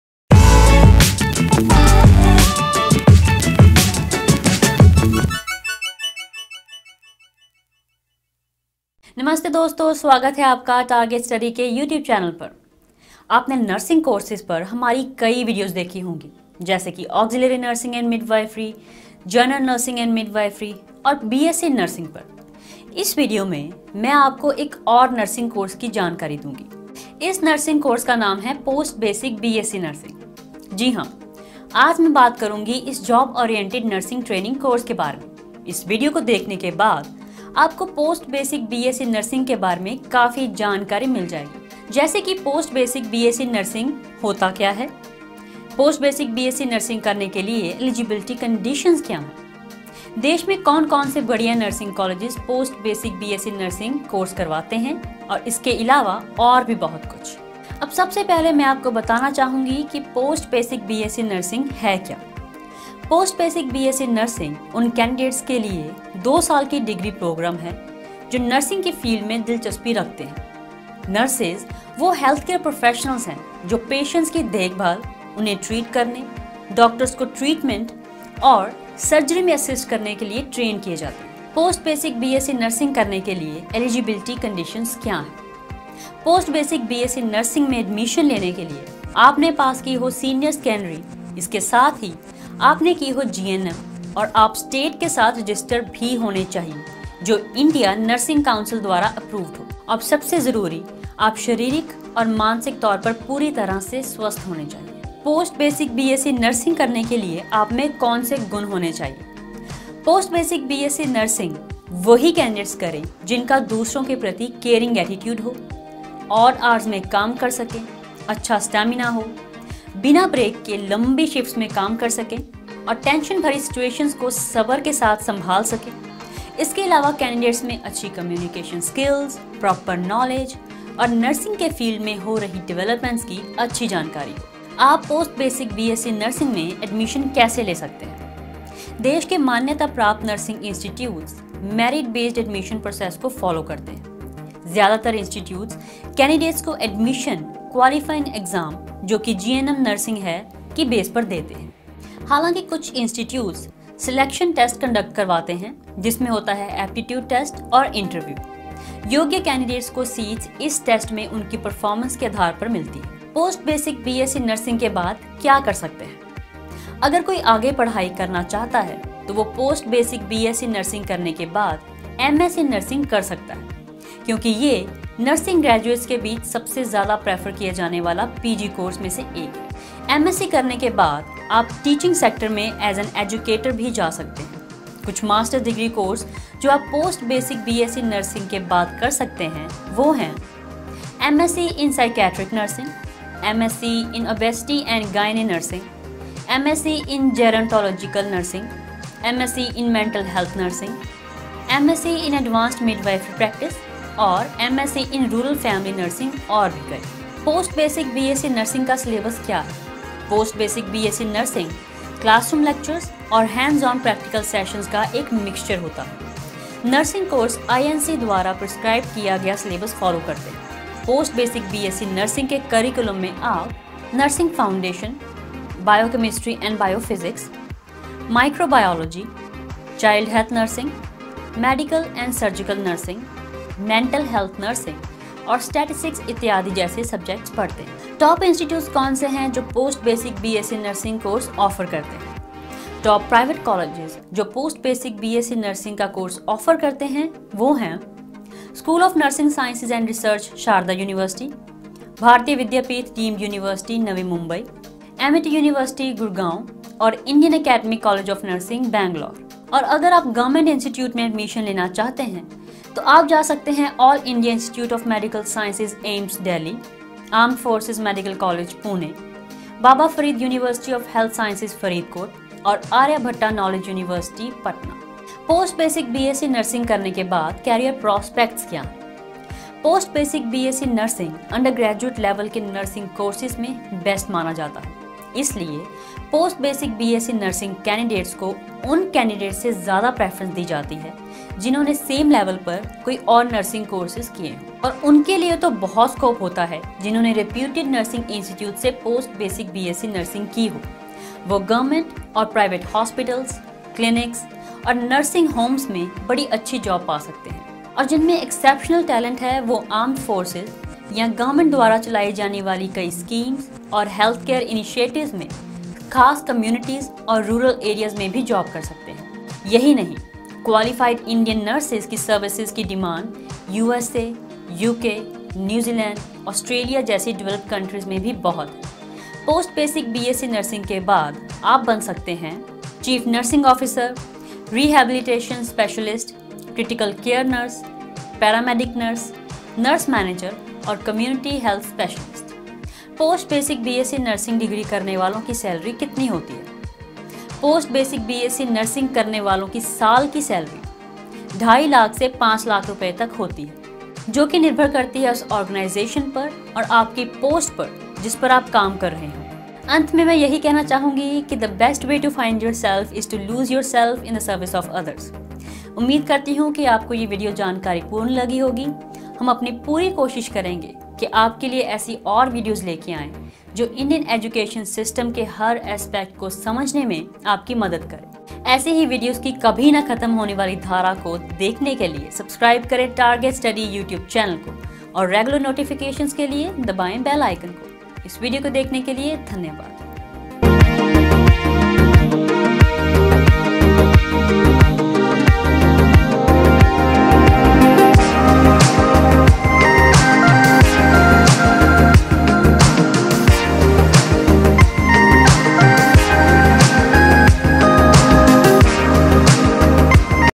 Post Basic B.Sc. Nursing | पोस्ट बेसिक बीएससी नर्सिंग | Its Eligibility, Syllabus, Colleges, Fee
Post Basic B.Sc nursing उन candidate’s के लिए 2 साल का degree program है, जो nursing के field में दिलचस्पी रखते हैं Nurses वह healthcare professionals हैं जो patients की देखभाल, उन्हें treat करने, doctors को treatment और surgery में assist करने के लिए trained किये जाते हैं.

Post Basic B.Sc Nursing Eligibility (शैक्षणिक योग्यता):
Post Basic B.Sc Nursing में admission लेने के लिए आपने पास की हो senior secondary इस के साथ ही आपने की हो GNM और आप state के साथ register भी होने चाहिए जो India Nursing Council द्वारा approved हो और सबसे जरूरी आप शारीरिक और मानसिक तौर पर पूरी तरह से स्वस्थ होने चाहिए।

Post Basic B.Sc. Nursing करने के लिए आप में कौन से गुण होने चाहिए?
देश के मानयता प्राप्त nursing institutes Merit-based admission process को follow करते हैं. ज्यादा तर institutes candidates को admission qualifying exam जो की GNM nursing है के base पर देते हैं हालाँकि कुछ institutes selection test conduct करवाते हैं जिसमे होता है aptitude test और interview. योग्य candidates को seats, इस test में उनकी performance के आधार पर मिलती है.

Post Basic B.Sc Nursing के बाद क्या कर सकते हैं?
अगर कोई आगे पढ़ाई करना चाहता है तो वह Post Basic B.Sc Nursing करने के बाद M.Sc Nursing कर सकता है क्यों की यह nursing graduates के बीच सबसे ज्यादा prefer किए जाने वाले PG courses में से  एक है M.Sc. करने के बाद आप teaching sector में as an educator भी जा सकते हैं कुछ master degree courses जो आप

Post Basic B.Sc Nursing के बाद कर सकते हैं वो हैं:
• M.Sc in Psychiatric Nursing
• M.Sc in OBST and Gyane Nursing
• M.Sc in Gerontological Nursing
• M.Sc in Mental Health Nursing
• M.Sc in Advanced Midwifery Practice and
• M.Sc in Rural Family Nursing और भी कई

Post Basic B.Sc Nursing का Syllabus क्या है?
Post Basic B.Sc Nursing classroom lectures और hands-on practical sessions का एक mixture होता है. Nursing Course INC द्वारा prescribed किया गया syllabus follow करते हैं Post Basic B.Sc. Nursing के curriculum में आप:
• Nursing foundation
• Biochemistry and Biophysics
• Microbiology
• Child Health Nursing
• Medical and Surgical Nursing
• Mental health nursing and
• Statistics etc… जैसे subjects पढ़तें हैं

Top Institutes कौन से हैं जो Post Basic B.Sc. Nursing Course Offer करते हैं?
Top private colleges जो post basic B.Sc nursing का course offer करते हैं वह हैं:
• स्कूल ऑफ़ नर्सिंग साइंसेज एंड रिसर्च, शारदा यूनिवर्सिटी
• भारतीय विद्यापीठ डीम्ड यूनिवर्सिटी, नवी मुंबई.
• Amity यूनिवर्सिटी, गुडगाँव
• इंडियन अकादमी कॉलेज ऑफ़ नर्सिंग, बैंगलोर
और अगर आप government institute में admission लेना चाहते हैं तो आप जा सकते हैं:
• आल इंडिया इंस्टिट्यूट ऑफ़ मेडिकल साइंसेज - AIIMS, दिल्ली
• आर्म्ड फोर्सेज मेडिकल कॉलेज, पुणे
• बाबा फरीद यूनिवर्सिटी ऑफ़ हेल्थ साइंसेज, फरीदकोट एंड
• आर्यभट्ट नॉलेज यूनिवर्सिटी, पटना

Post Basic B.Sc. Nursing Course करने की Fee क्या है?
Post Basic B.Sc Nursing course की fee government institute में हो सकती है 20,000/- Rs से 30,000/- Rs  तक जबकि private colleges में इस course की फीस हो सकती है 1 लाख से 3 लाख तक

Post Basic B.Sc. Nursing करने के बाद Career Prospects क्या हैं?
Post Basic B.Sc Nursing, undergraduate level के nursing courses में best माना जाता है इसीलिए Post Basic B.Sc Nursing candidates को उन candidates से ज़ादा preference दी जाती है जिन्होंने same level पर कोई और nursing courses किया हो और उन के लिए तो बहुत scope होता है जिन्होंने reputed nursing institutes से Post Basic B.Sc Nursing की हो वह government और private hospitals, clinics और nursing homes में बड़ी अच्छी job मिल सकती है और जिनमे exceptional talent है वो Armed forces या government द्वारा चलाई जाने वाली कई schemes और healthcare initiatives में, ख़ास communities और rural areas में भी job कर सकतें हैं यही नहीं qualified Indian Nurses की services की demand USA, UK, New Zealand, Australia जैसी developed countries में भी बहुत है.

Post Basic B.Sc Nursing करने के बाद आप बन सकते हैं:
• Chief Nursing Officer
• Rehabilitation Specialist
• Critical Care Nurse
• Paramedic Nurse
• Nurse Manager and
• Community Health Specialist

Post Basic B.Sc Nursing Degree करने वालों की कितनी Salary होती है?
Post Basic B.Sc Nursing करने वालों की साल की salary हो सकती है 2.5 लाख से 5 लाख Rs तक जबकि यह पूरी तरह से निर्भर करती है organization आपके post पर जहाँ वह काम कर रहा हो और जो organization उन्होंने join किया हो.